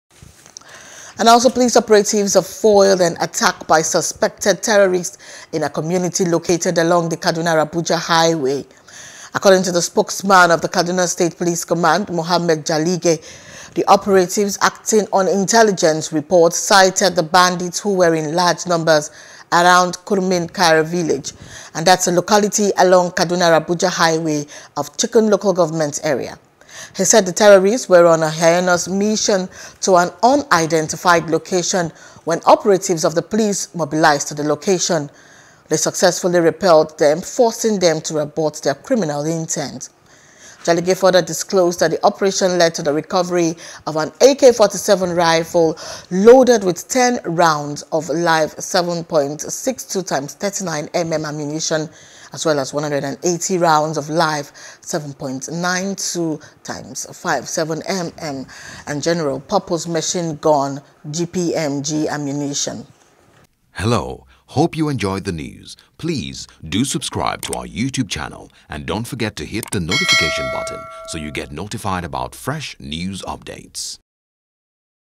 Security: Police Repel Bandits On Kaduna-Abuja Highway | NEWS
Police operatives have foiled an attack by suspected terrorists in a community located along the Kaduna-Abuja Highway.